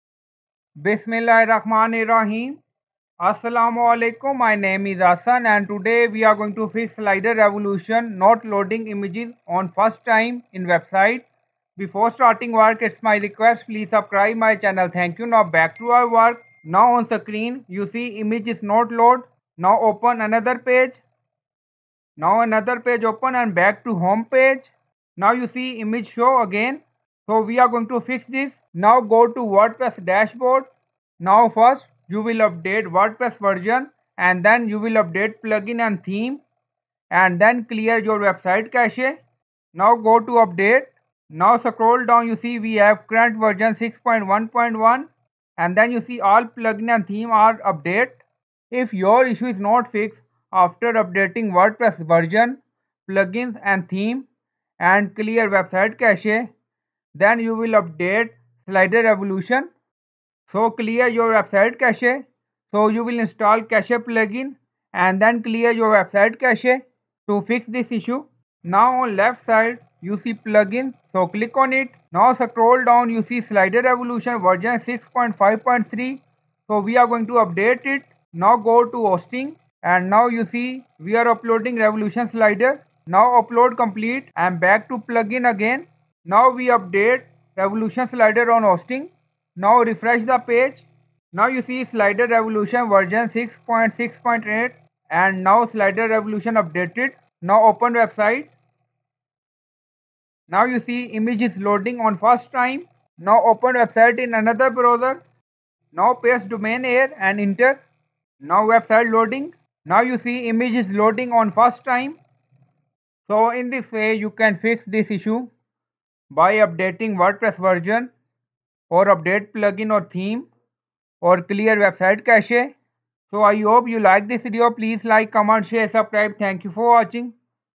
How to Fix Slider Revolution not Loading Images on First Time in WordPress
In this slider revolution tutorial you will learn how to fix issue revolution slider not show image on first time when we open / visit wordpress website first time. If your slider plugin isn't updated or doesn't match to your WordPress version, it can also cause a problem with the images. So you will update wordpress version, update all plugins and theme and remove / clear website cache and also update revolution slider plugin.